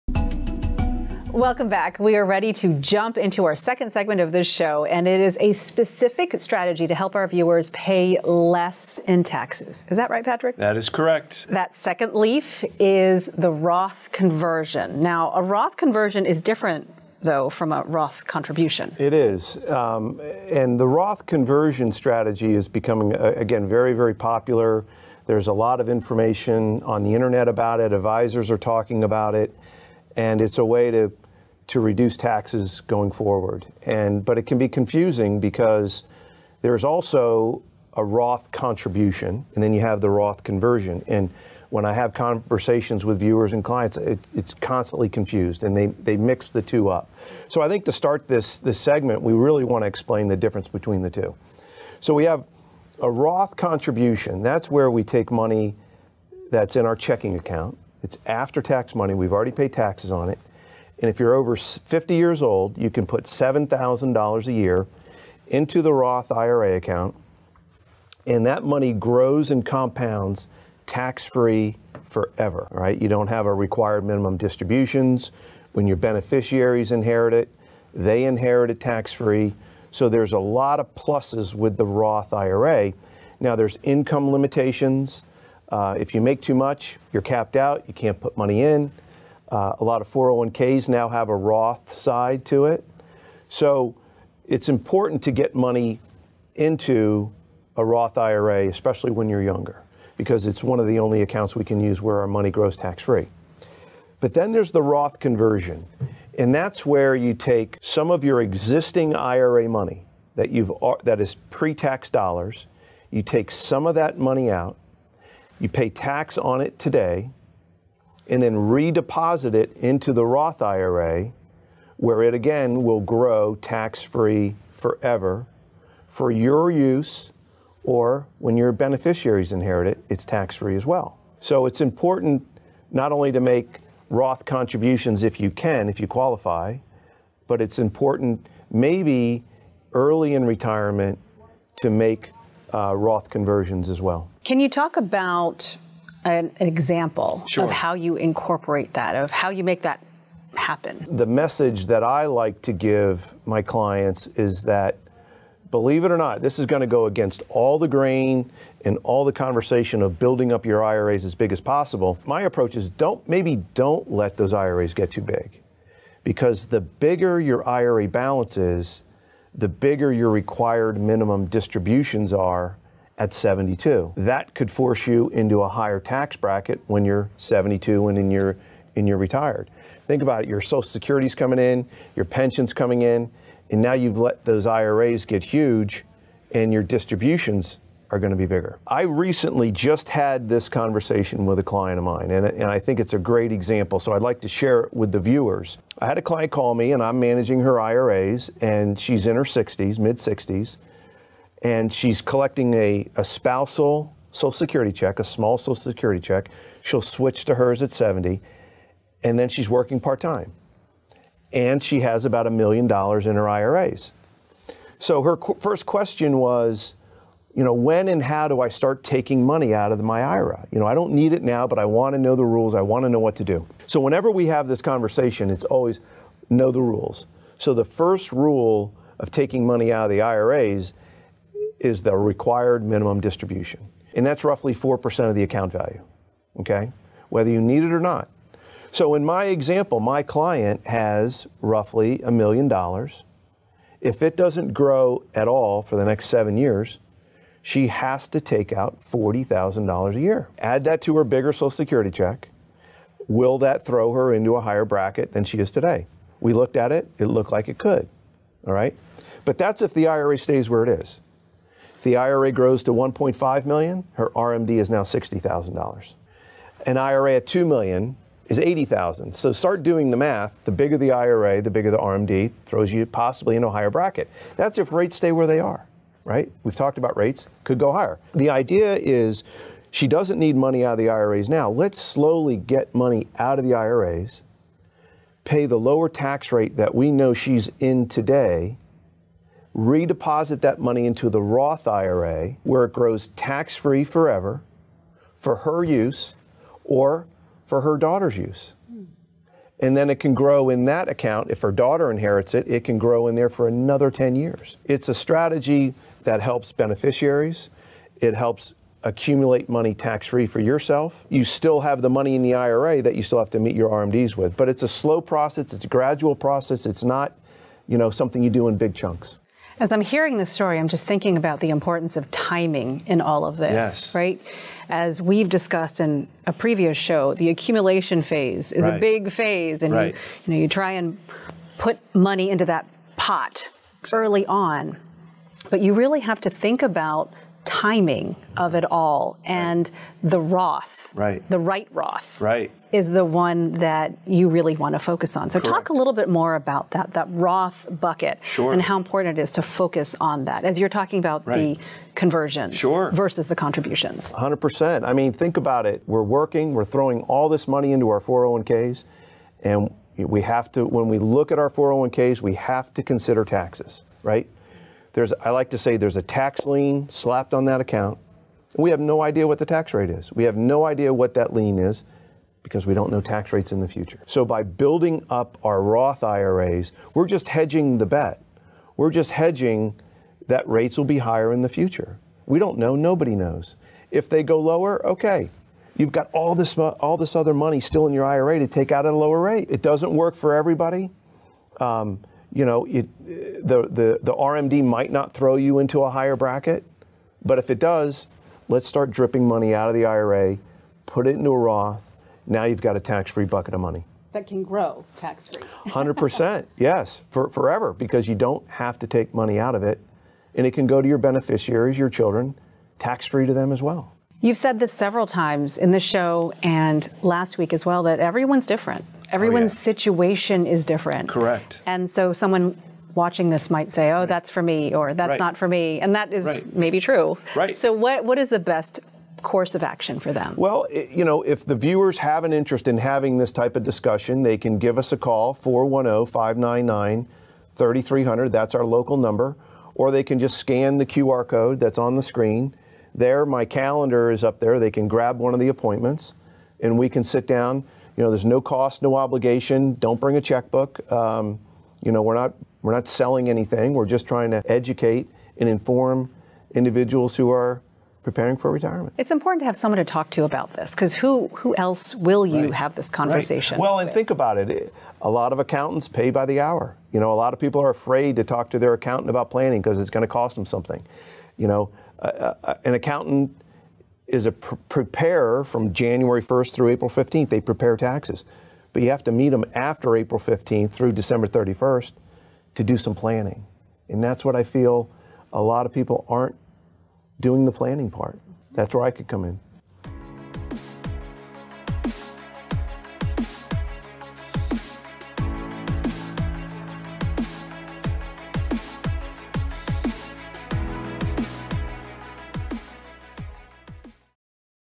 Contributions and Conversions – Understanding the World of Roth
We’re all about helping our clients hold on to as much of their 𝗵𝗮𝗿𝗱-𝗲𝗮𝗿𝗻𝗲𝗱 𝗺𝗼𝗻𝗲𝘆 as possible. 💵

One of the strategies we use to mitigate tax liability are 𝗥𝗼𝘁𝗵 𝗜𝗥𝗔𝘀, which you can convert into or contribute towards.

𝗖𝗼𝗻𝘁𝗿𝗶𝗯𝘂𝘁𝗶𝗼𝗻𝘀 are made with after-tax dollars and are capped on an annual basis based on what you earn. 𝗖𝗼𝗻𝘃𝗲𝗿𝘀𝗶𝗼𝗻𝘀 happen when you take existing pre-tax IRA money, take the current tax hit, and redeposit it into a Roth IRA where it can grow TAX-FREE! 🙌

Of course, you want to be weary as to not bump yourself up into another tax bracket, so 𝗯𝗲𝘀𝘁 𝘀𝘁𝗿𝗮𝘁𝗲𝗴𝗶𝘇𝗲 𝘄𝗶𝘁𝗵 𝗼𝘂𝗿 𝗽𝗿𝗼𝗳𝗲𝘀𝘀𝗶𝗼𝗻𝗮𝗹𝘀 here at Moriarty Financial Services first!

Use the timestamps below to jump to a section! ⬇️

𝗧𝗜𝗠𝗘𝗦𝗧𝗔𝗠𝗣𝗦
00:00 Limit tax liability with Roth accounts
00:51 The difference between contribution and conversion
02:31 Don’t let IRAs get too big
05:25 The importance of timing
07:15 Give our team a call to discuss if a Roth conversion is right for you

Do you have specific questions you would like to address with Patrick directly? Book a 𝗰𝗼𝗺𝗽𝗹𝗶𝗺𝗲𝗻𝘁𝗮𝗿𝘆 𝘃𝗶𝘀𝗶𝘁 here 👇